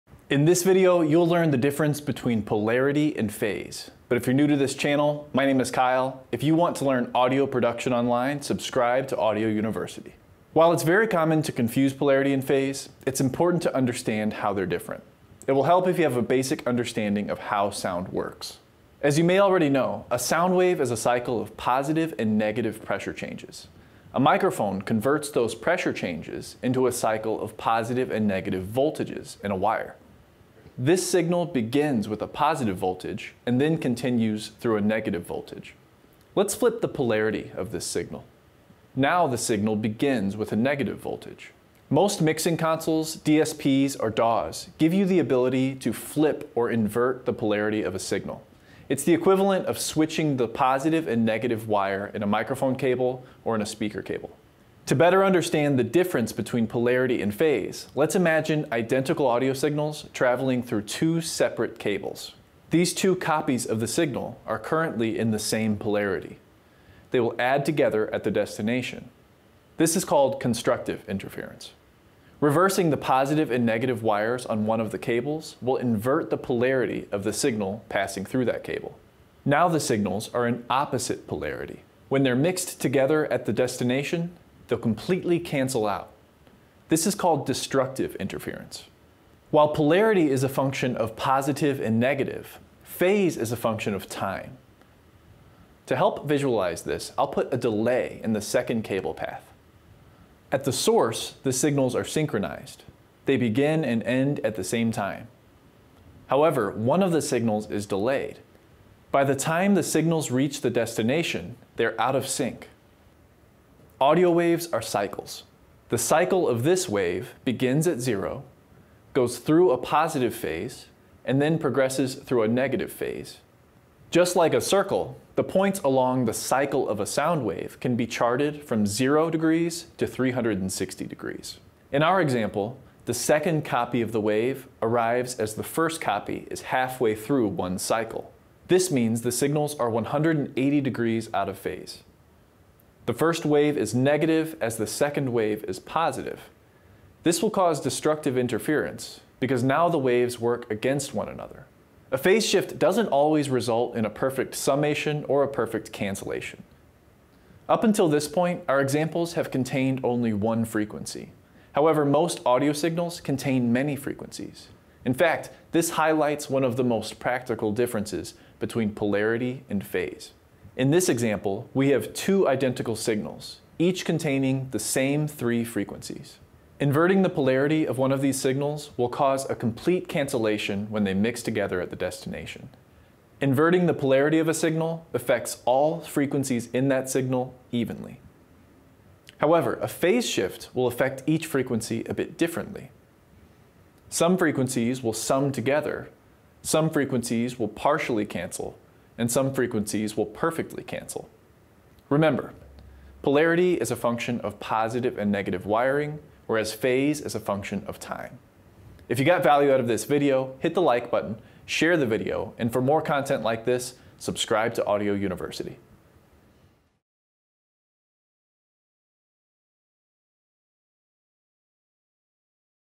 POLARITY vs PHASE: What's the Difference?
It's very common to confuse the polarity and phase of an audio signal. In this video, you'll learn what makes polarity and phase different and why it's important to understand the difference.

Need help setting up your speaker system? Check out this free resource I've created for you...

0:00 - Introduction
0:10 - Audio Basics Review
0:30 - What Is Polarity?
0:54 - Constructive and Destructive Interference
1:29 - What Is Phase?
2:37 - Polarity & Phase Shift: Multiple Frequencies
3:28 - Subscribe to Audio University!